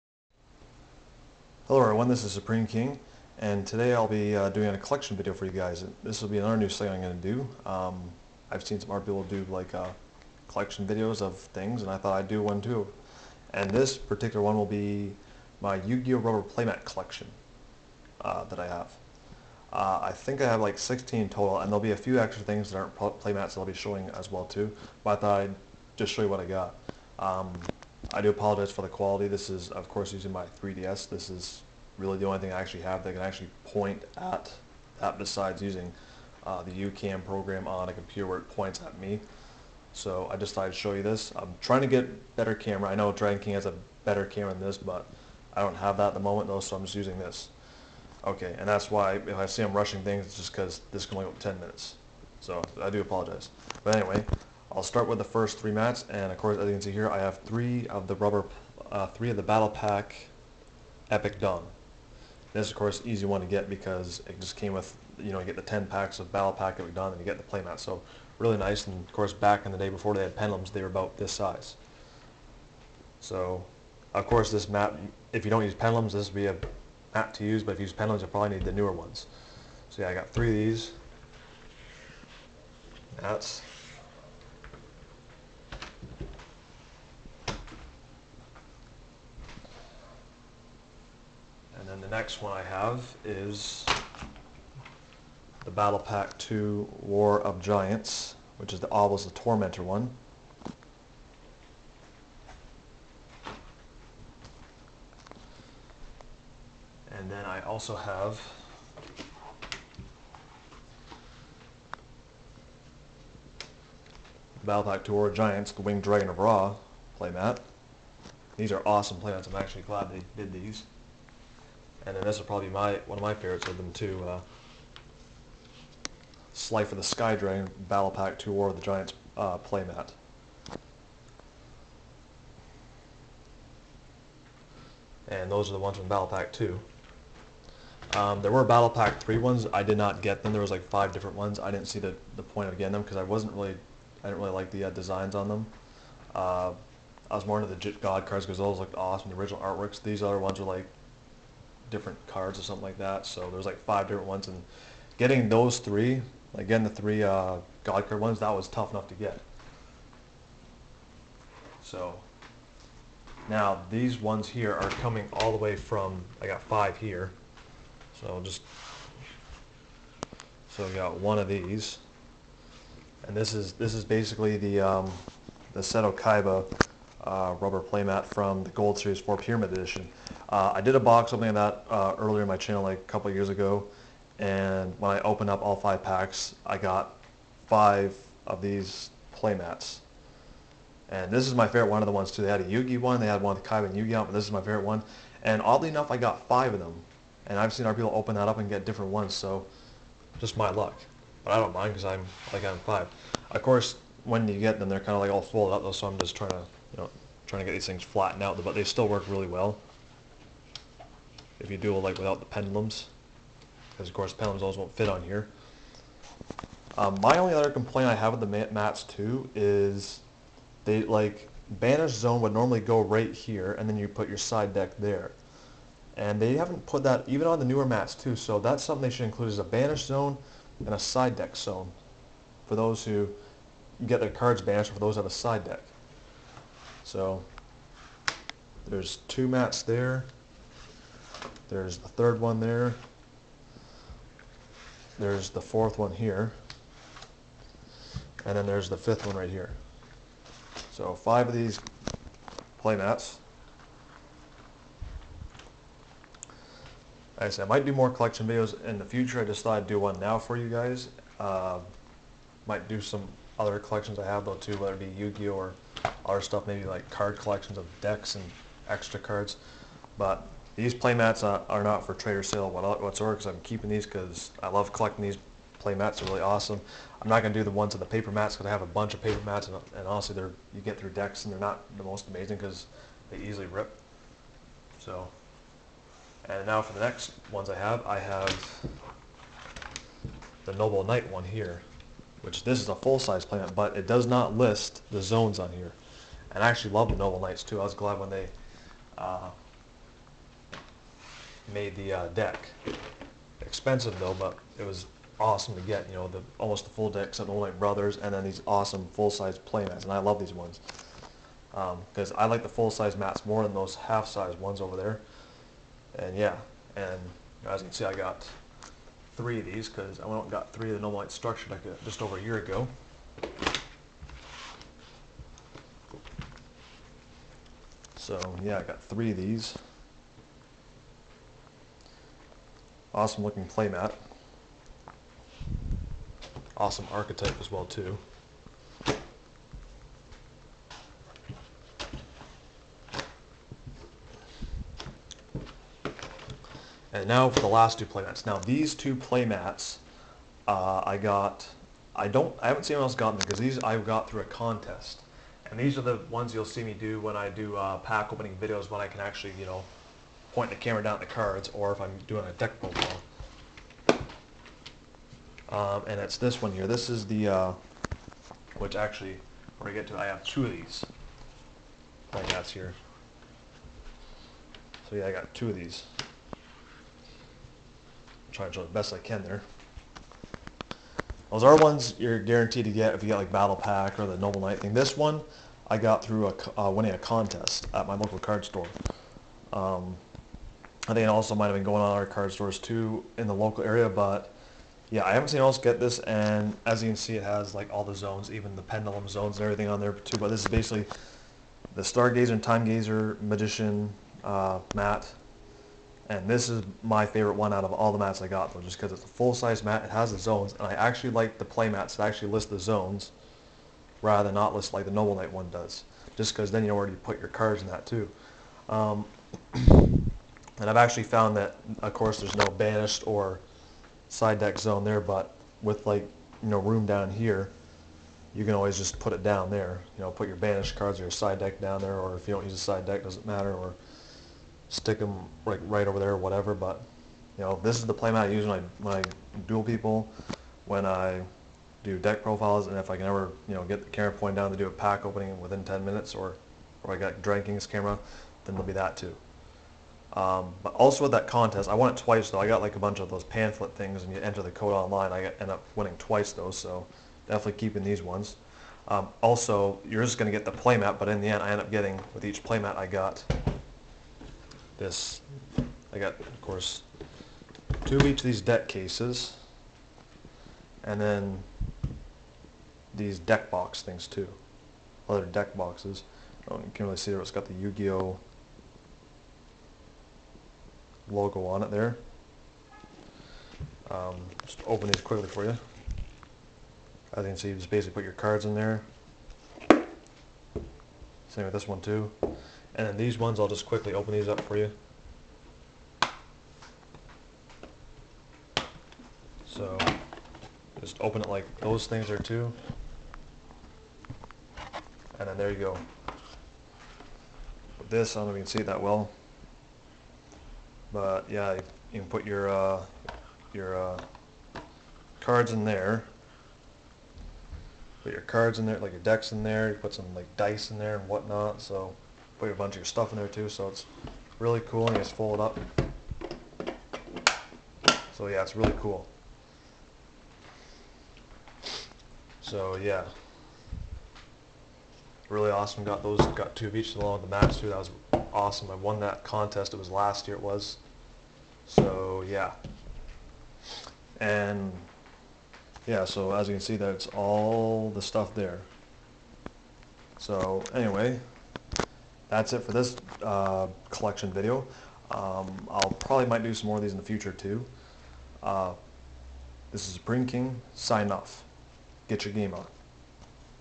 Yu-Gi-Oh! Rubber Play Mat Collection
This is my collection of Yu-Gi-Oh! rubber play mats.